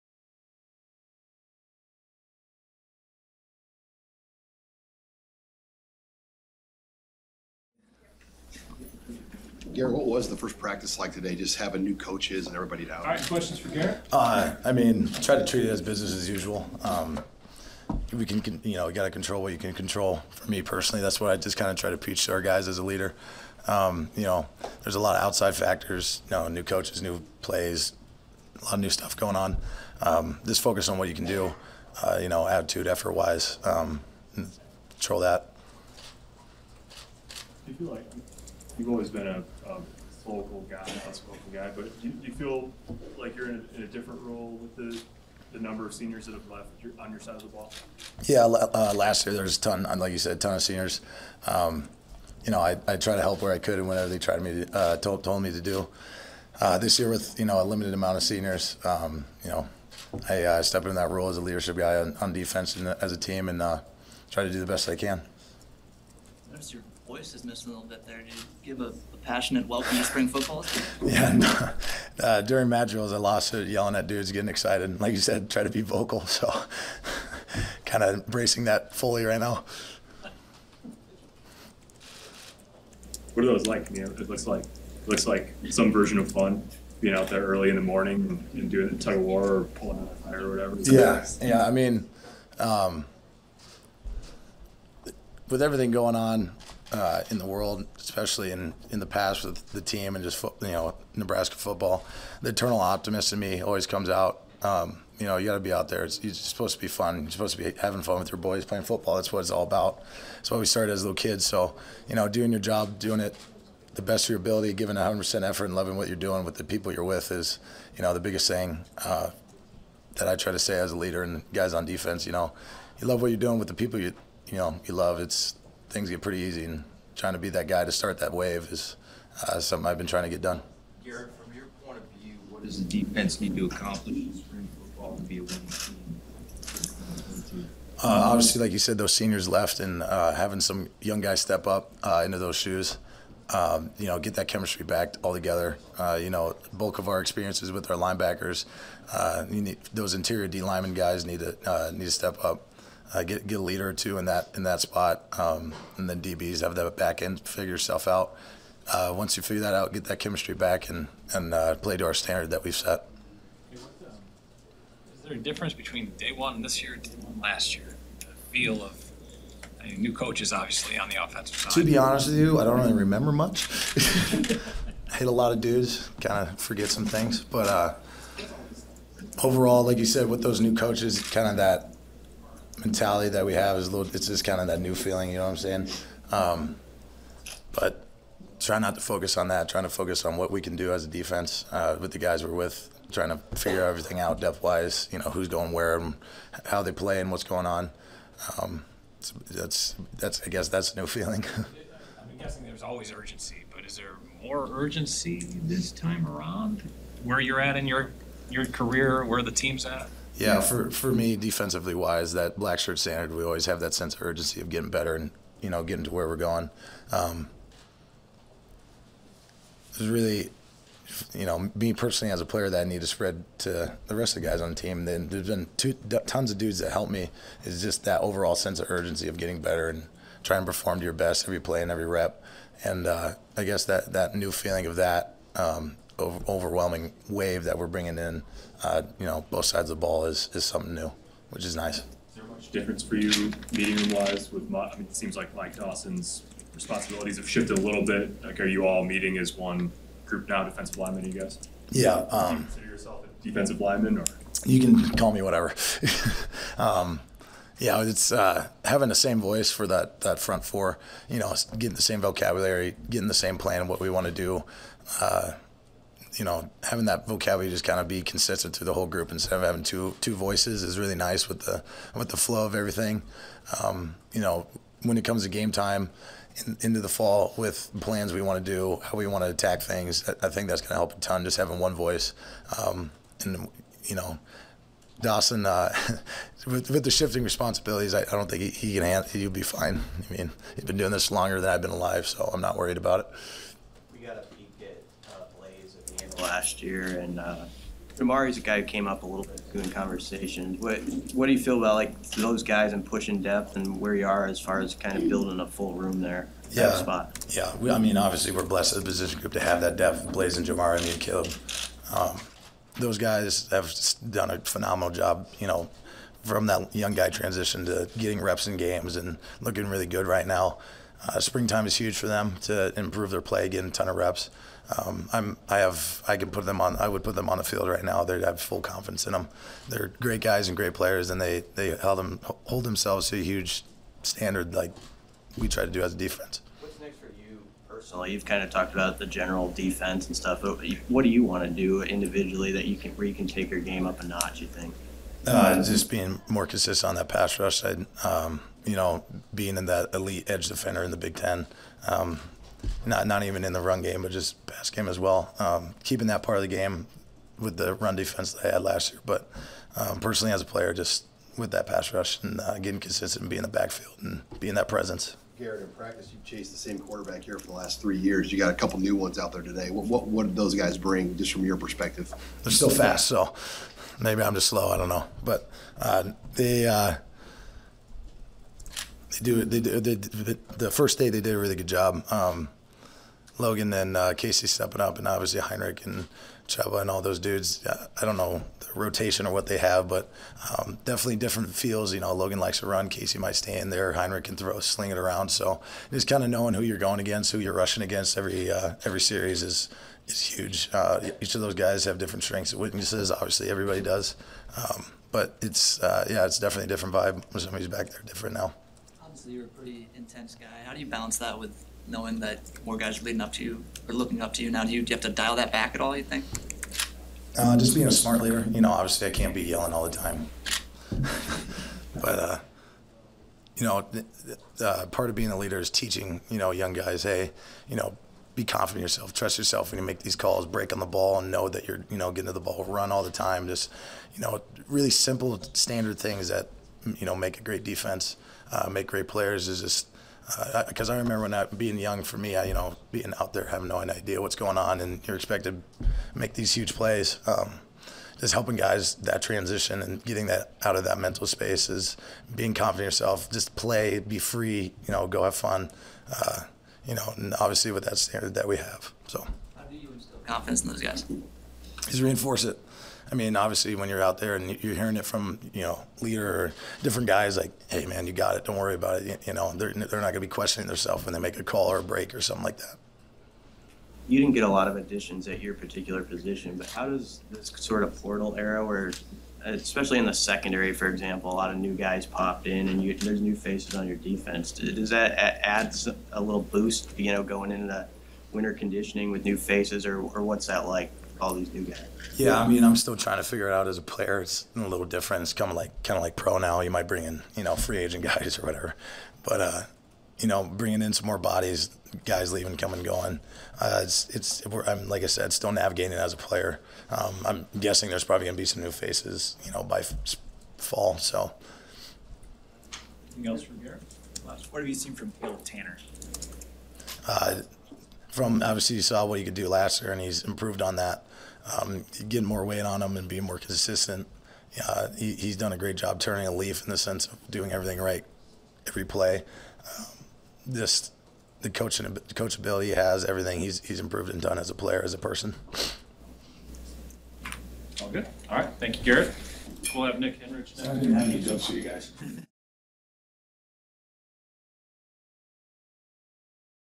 Watch now: A raspy-voiced Nelson describes his vocal leadership style
Nebraska's Garrett Nelson speaks during a news conference on Monday, Feb. 28, at Memorial Stadium.